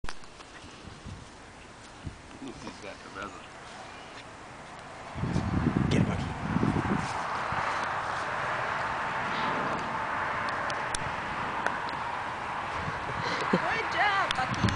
Bucky retrieving - 6 weeks
Age 6 weeks:  Bucky's first retrieve of pheasant wing